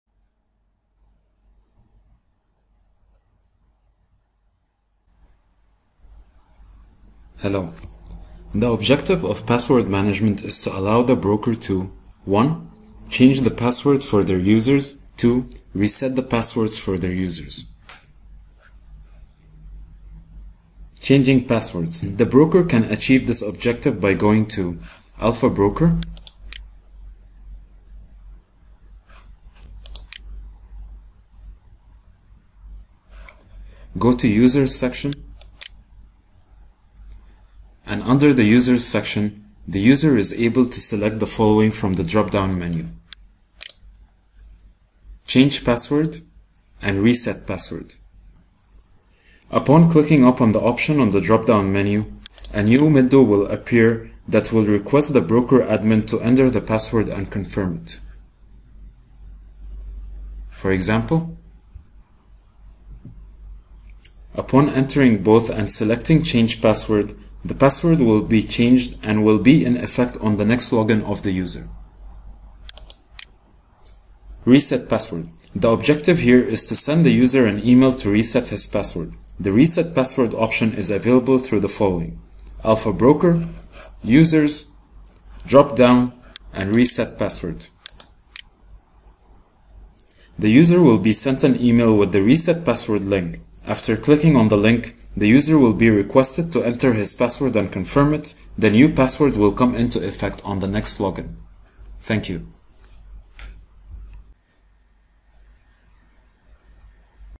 19 - Password Management
This video illustrated how to change a user password and how to reset the password for a specific user.